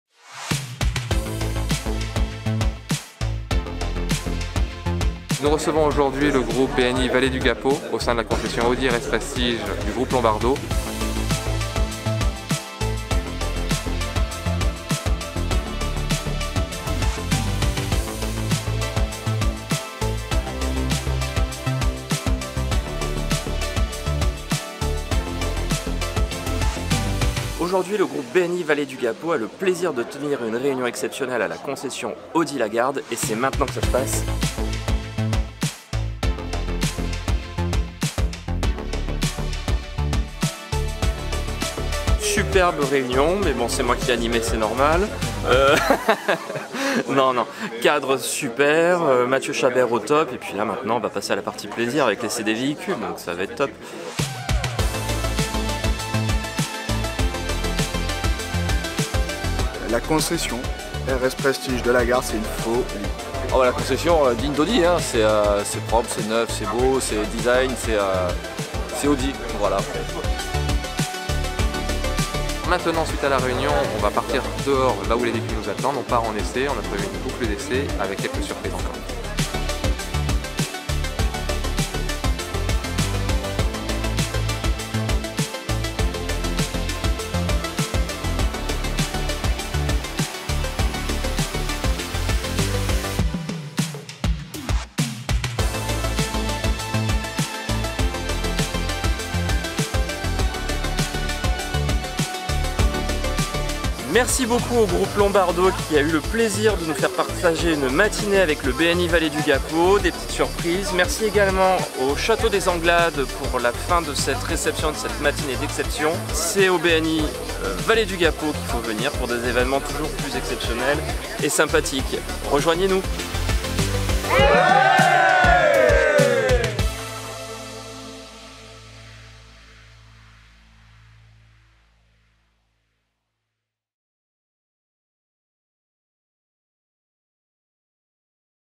Réunion BNI RS Prestige 2022
After movie d'une réunion BNI Vallée du Gapeau au sein de concession RS Prestige Audi à La Garde avec essai de véhicules et dégustation de vin au Château des Anglades.